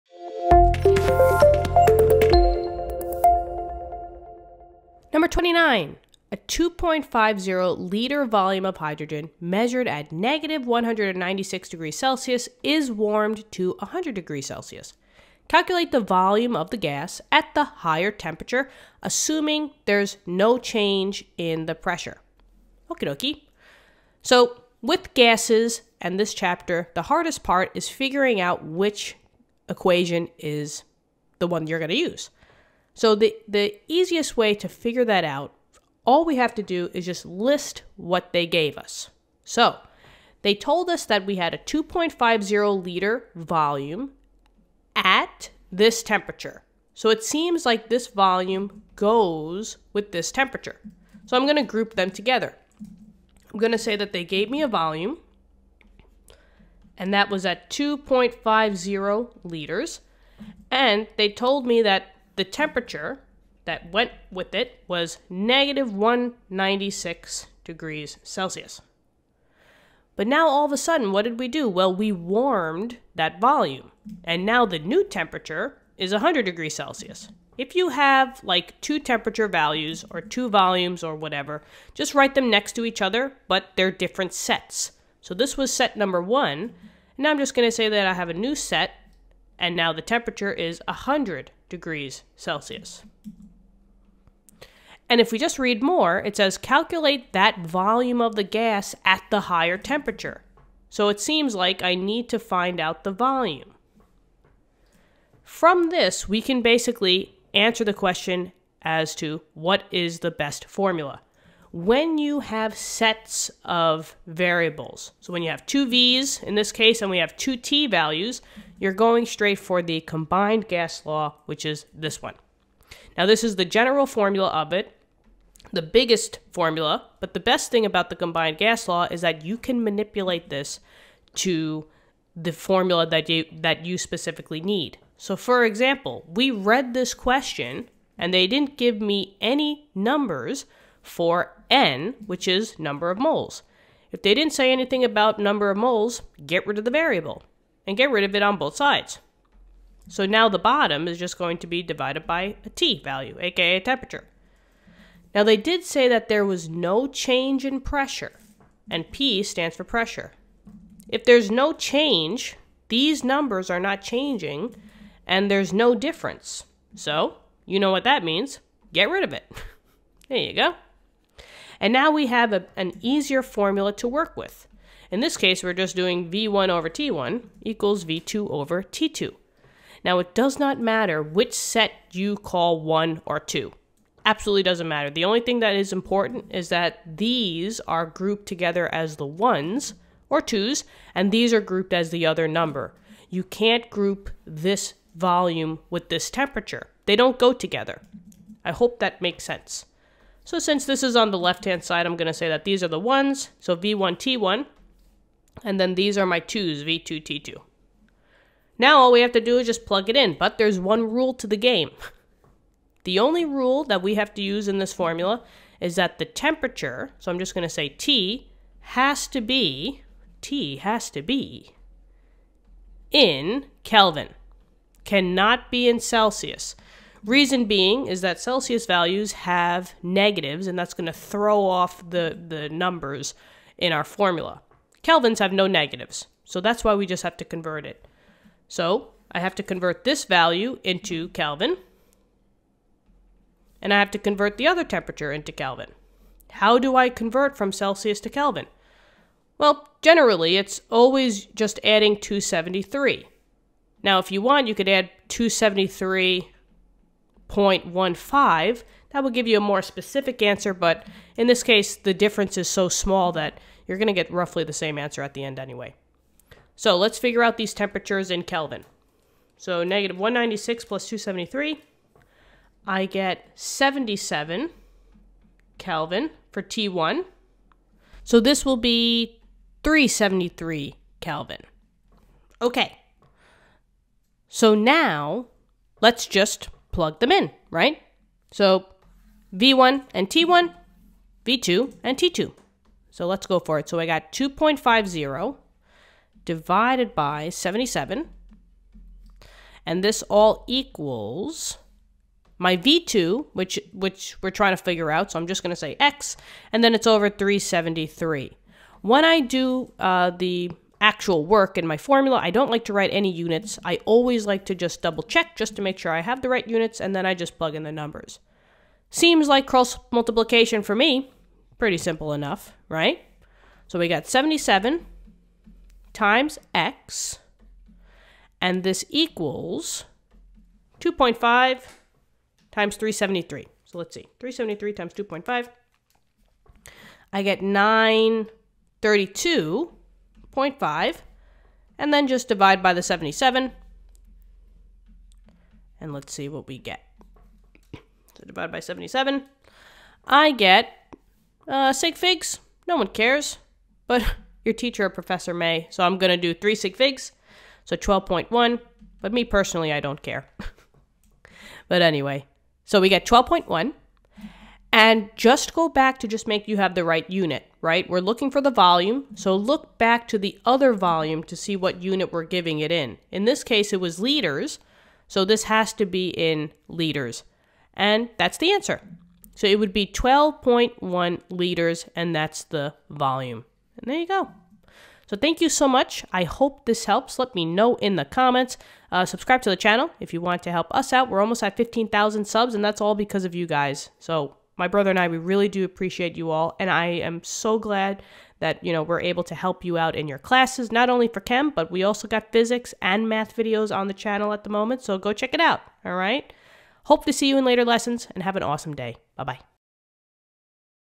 9.29 | A 2.50-L volume of hydrogen measured at –196 °C is warmed to 100 °C. Calculate the volume of
A 2.50-L volume of hydrogen measured at –196 °C is warmed to 100 °C. Calculate the volume of the gas at the higher temperature, assuming no change in pressure.